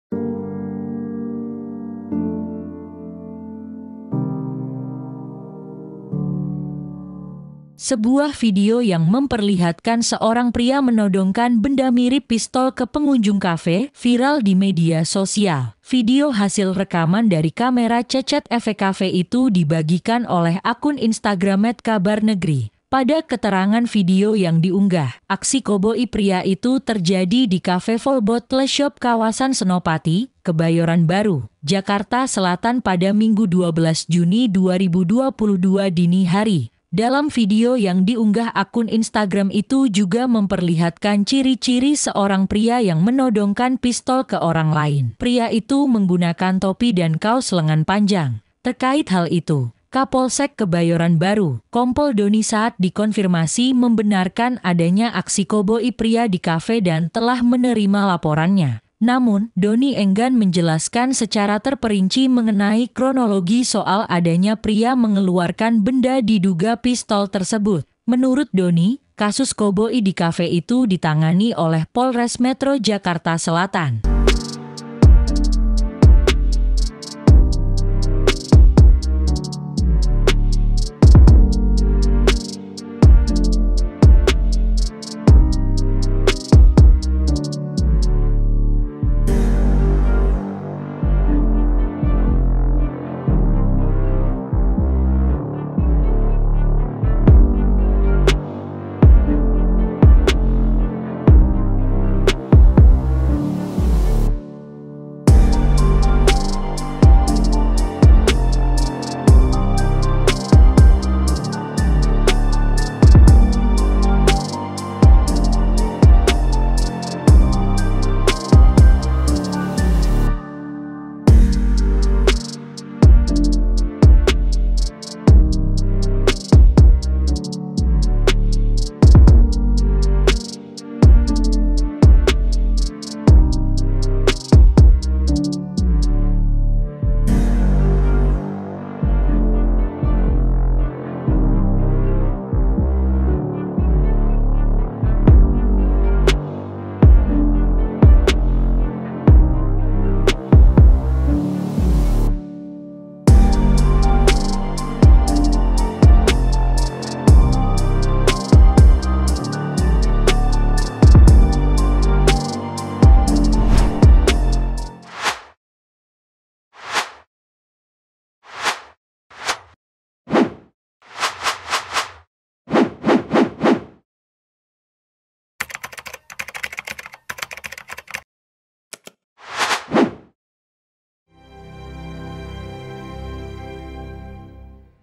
🔴 Viral Detik-detik Aksi Koboi Pria Todongkan Pistol ke Pengunjung Kafe di Senopati
Video hasil rekaman dari kamera CCTV kafe itu dibagikan oleh akun Instagram kabarnegri. Pada keterangan video yang diunggah, aksi koboi pria itu terjadi di Kafe Vol Bottle Shop kawasan Senopati, Kebayoran Baru, Jakarta Selatan pada Minggu 12 Juni 2022 dini hari.

Dalam video yang diunggah akun Instagram itu juga memperlihatkan ciri-ciri seorang pria yang menodongkan pistol ke orang lain.

Pria itu menggunakan topi dan kaos lengan panjang. Terkait hal itu, Kapolsek Kebayoran Baru, Kompol Doni saat dikonfirmasi membenarkan adanya aksi koboi pria di kafe dan telah menerima laporannya.

"Iya (sudah menerima laporan). Saat ini sedang ditangani Polres Jaksel," ujar Doni saat dihubungi, Rabu 15 Juni 2022.

Namun, Doni enggan menjelaskan secara terperinci mengenai kronologi soal adanya pria mengeluarkan benda diduga pistol tersebut. Menurut Doni, kasus koboi di kafe itu ditangani oleh Polres Metro Jakarta Selatan.